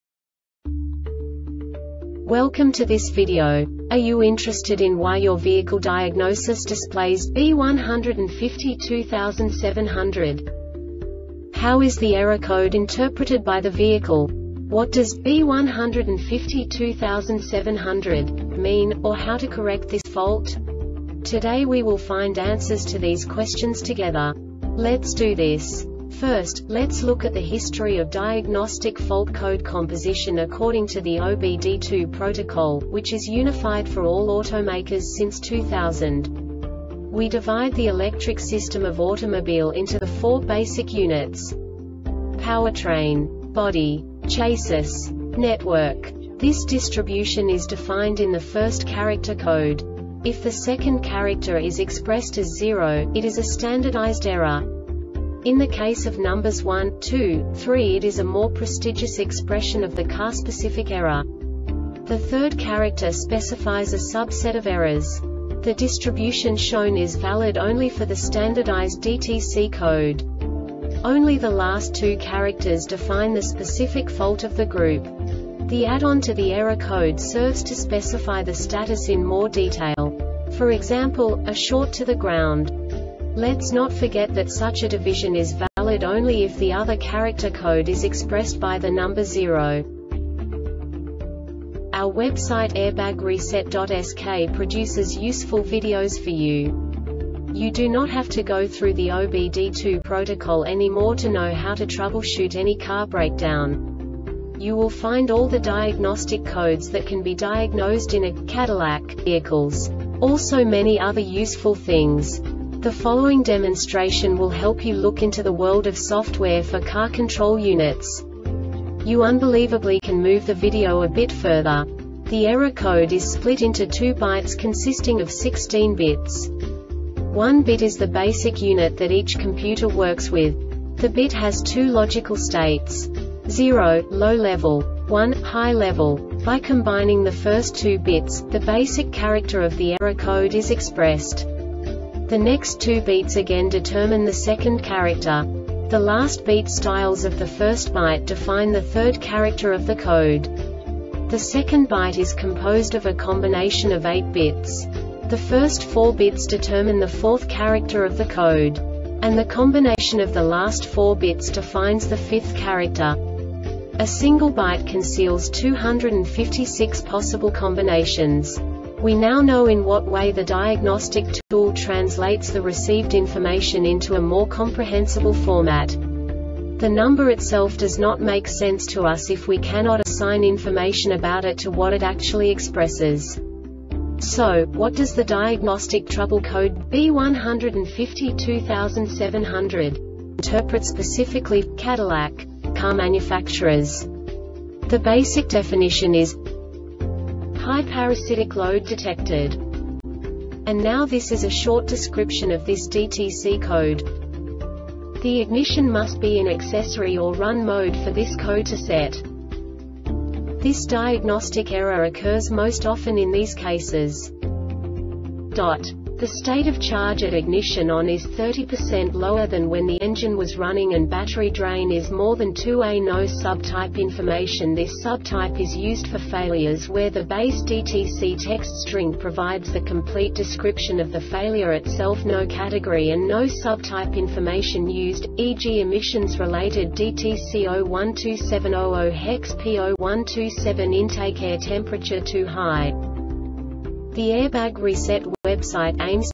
DTC cadillac B1527-00 Short Explanation
Contents:
0:21 Basic DTC analysis according to OBD2 protocol standard.
1:48 Insight into programming
2:58 A diagnostically understandable description of the Diagnostic Trouble Code
3:05 Basic error definition

Make:
Cadillac
Code:
B1527 00
Definition:
High Parasitic Load Detected
Description:
The ignition must be in Accessory or Run mode for this code to set.
Cause:
The state of charge at ignition ON is 30 percent lower than when the engine was running and battery drain is more than 2 A.